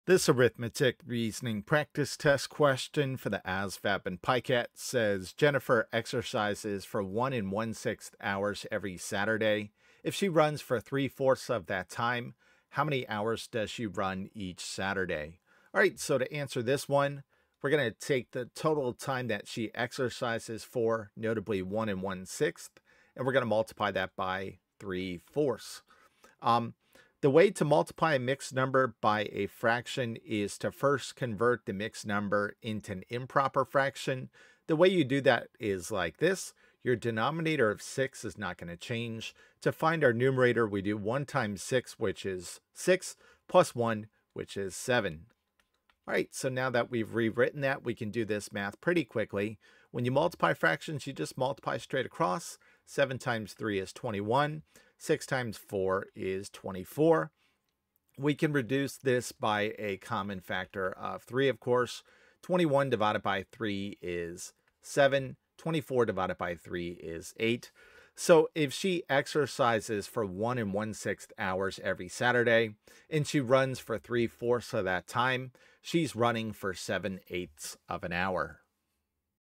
Multiplying Fractions: ASVAB/PiCAT Arithmetic Reasoning Practice Test Q
In this video, Grammar Hero works out a practice test question that requires you to quickly multiply fractions. This question should closely mirror what you should expect to see in the Arithmetic Reasoning (AR) subtest of  the  Armed Services Vocational Aptitude Battery (ASVAB) as well as the Pre-screening internet-delivered Computer Adaptive Test (PiCAT).